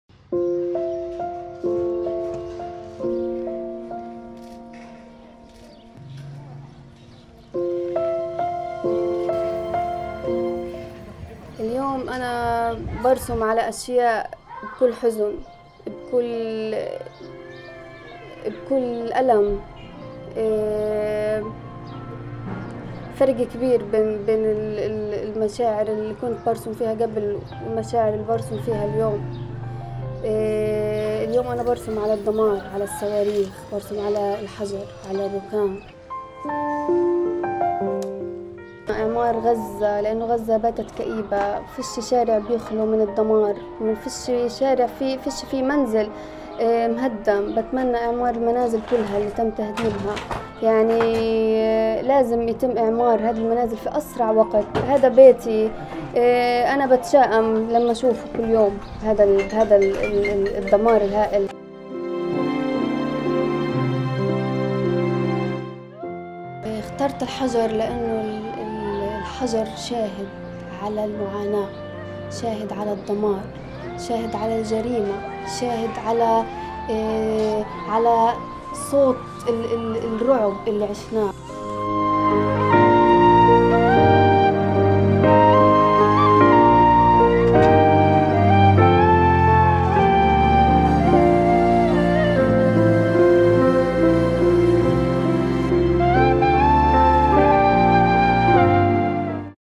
Gaza artist paints on the remains of her demolished house
Walking through piles of rubble and debris, 26-year-old Gazan artist Saja Mousa, assessed the damage to her house hit by Israeli airstrikes around a month ago.

She picked up pieces of stone and searched for shrapnel and missiles to turn them into pieces of art.

Using green, red, white and black, almost all of her art pieces adorned the colors of the Palestinian flag.